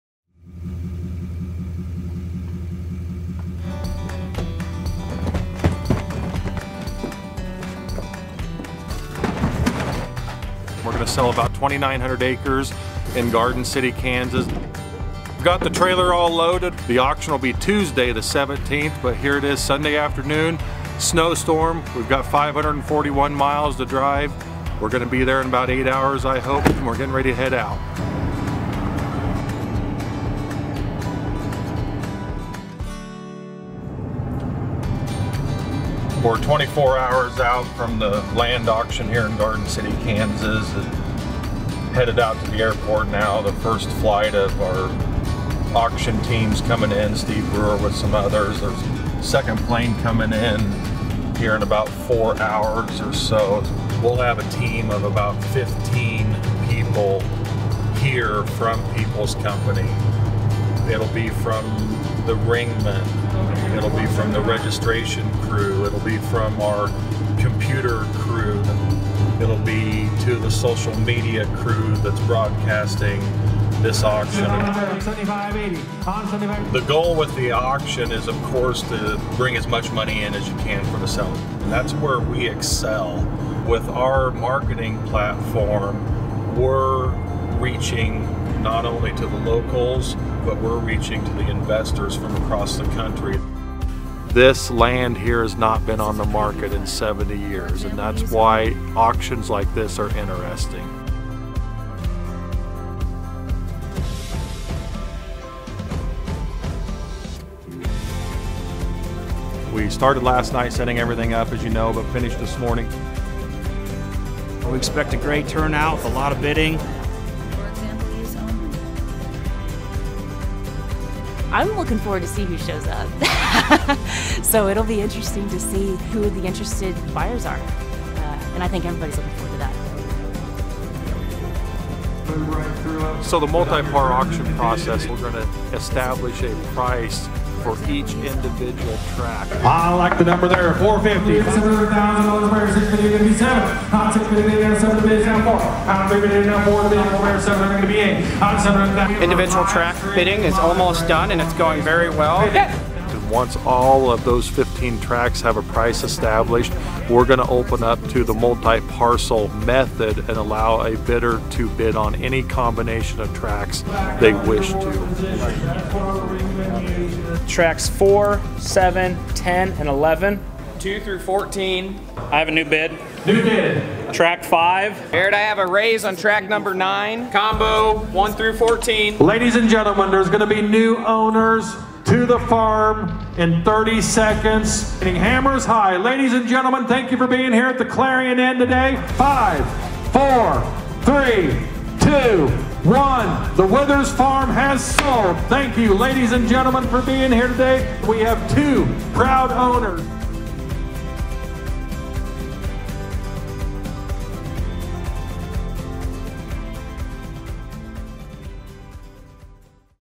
Haskell County, Kansas Auction
The Withers Farm is located in Haskell County, Kansas 25 miles from Garden City, Kansas. The property was offered in 15 tracts ranging from 1.1 acres to 460 acres m/l via the multi-parcel auction format where tracts can be purchased individually or any combination. The Farm is comprised of 2,898 acres m/l and sold “Absolute” to the highest bidder. The property consists of 2,813.28 FSA tillable acres m/l that carry an NCCPI of 58. Primary soils include the highly productive Richfield Silt Loam and Ulysses Silt Loam. The Withers Farm has numerous improvements including 556,000 bushels of grain storage, a 4,999 head feedlot and multiple dwellings. The Withers family has built extensive water rights over the years which authorize approximately 2,475 acres m/l to be irrigated through 16 well-maintained pivots. Property is ideally situated between Garden City and Dodge City with quick access to Highway 56 and Highway 83.